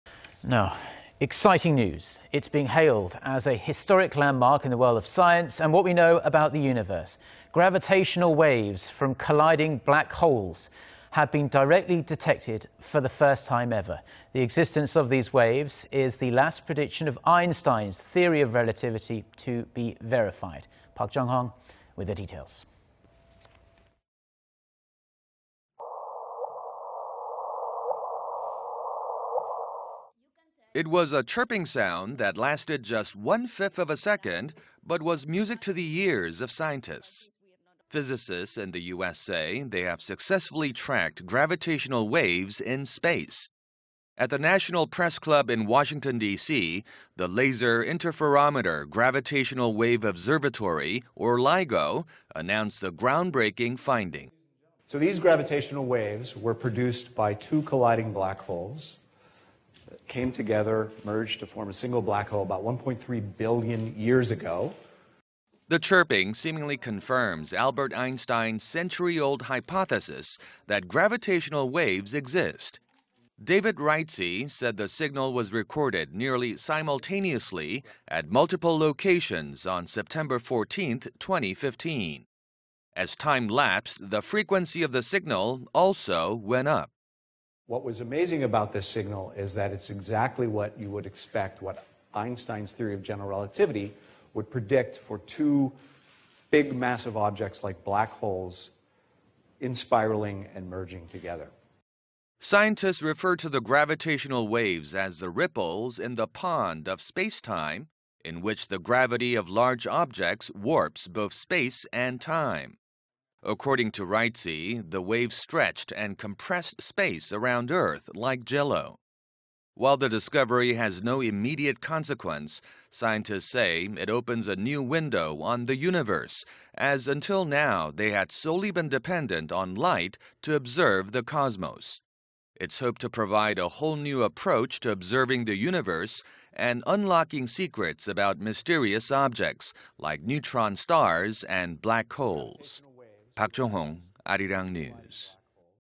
Physicists confirm existence of gravitational waves
아인슈타인 주장 '중력파' 직접 탐지 성공
It's being hailed as a historic landmark in world of science and what we know about the universe.
Gravitational waves from colliding black holes have been directly detected for the first time.
The existence of these waves is the last prediction of Einstein's theory of relativity to be verified.
Park Jong-hong with the details.

It was a chirping sound that lasted just one-fifth of a second but it was music to the ears of scientists.
Physicists in the U.S. say they have successfully tracked gravitational waves in space.
At the National Press Club in Washington D.C., the Laser Interferometer Gravitational-Wave Observatory, or LIGO announced its groundbreaking finding.

"So these gravitational waves were produced by two colliding black holes that came together merged to form a single black hole about 1.3 billion years ago."

The chirping seemingly confirms Albert Einstein's century-old hypothesis that gravitational waves exist.
David Reitze said the signal was recorded nearly simultaneously at multiple locations on September 14th, 2015.
As time lapsed, the frequency of the signal also went up.

"What was amazing about this signal is that it's exactly what you would expect what Einstein's theory of general relativity would predict for two big massive objects like black holes in spiraling and merging together."

Scientists refer to the gravitational waves as the ripples in the pond of spacetime in which the gravity of large objects warps both space and time.
According to Rietze,... the waves stretched and compressed space around Earth "like Jell-O".
While the discovery has no immediate consequence, scientists say it opens a new window on the universe as until now they had solely been dependent on light to observe the cosmos.
It's hoped to provide a whole new approach to observing the universe and unlocking secrets about mysterious objects like neutron stars and black holes.
Park Jong-hong, Arirang News.